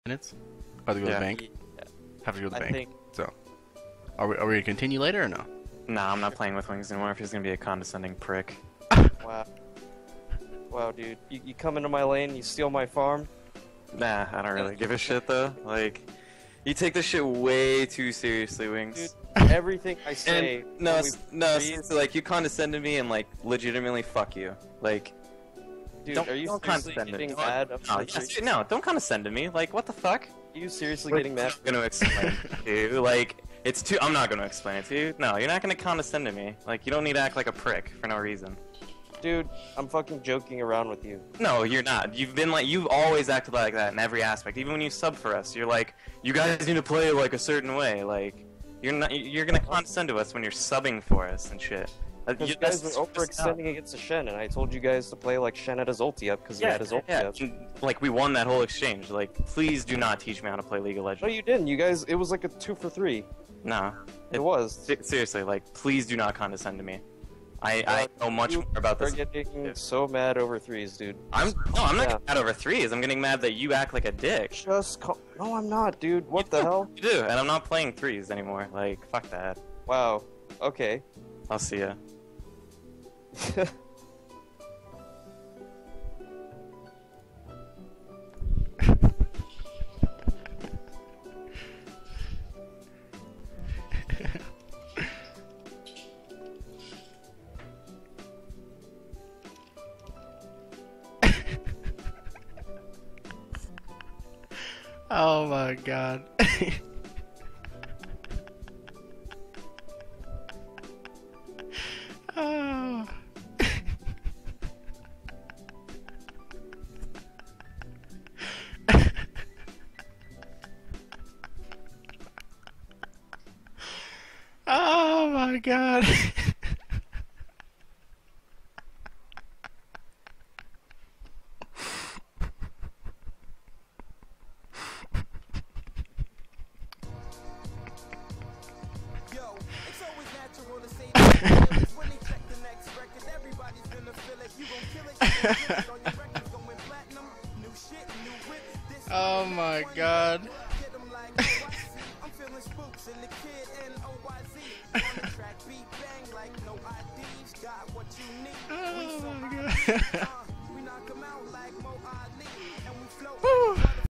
The End of the Three Stooges
MuffinQT and Wingsofdeathx hash out their love for each other after a rough 3's game.